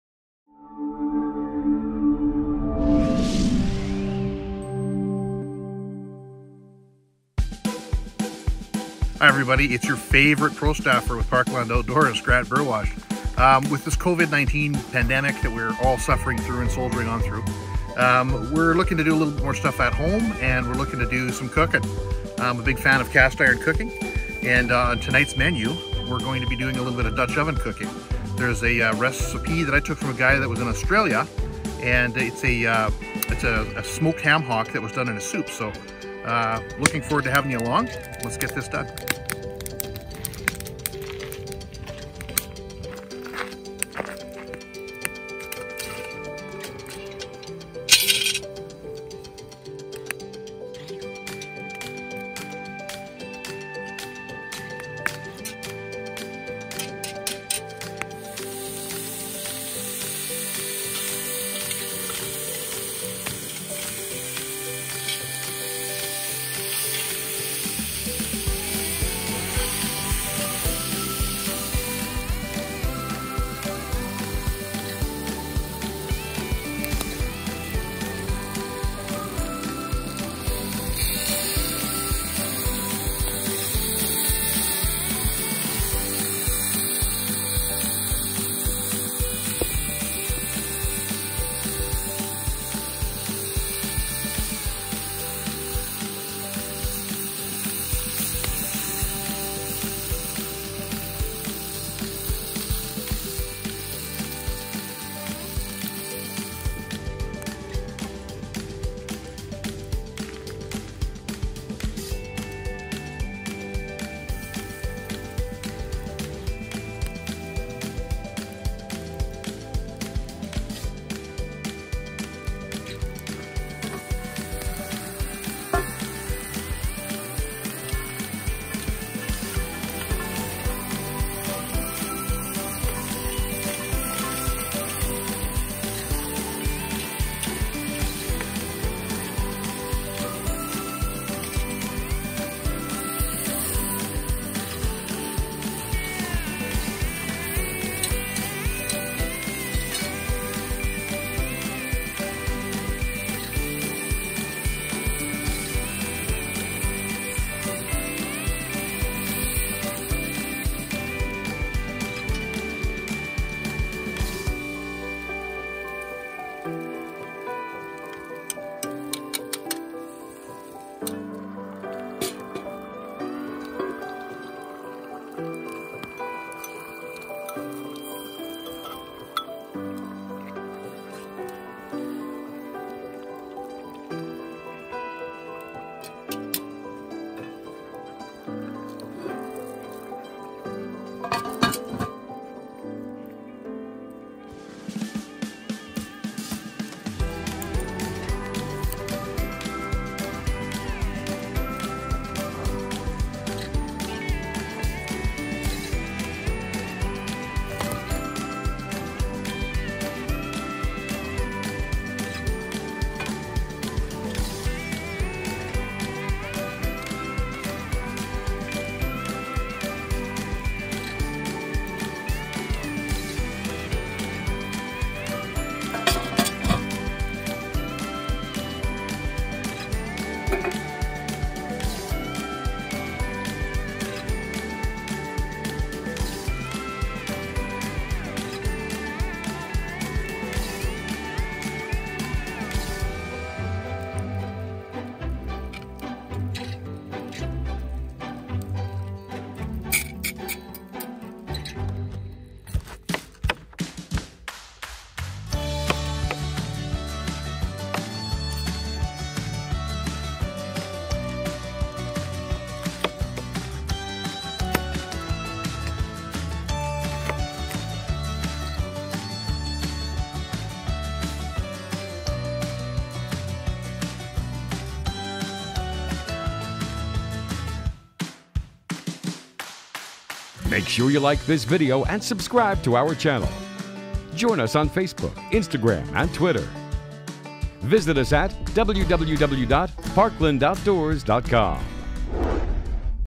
Dutch Oven Cookout - Parkland Outdoors - Cooking & BBQ
Your favorite prostaffer, Grant Burwash, takes us to his backyard fire pit for some cowboy cooking.  This week he's preparing smoked ham hock soup in a dutch oven and ribs on the grill.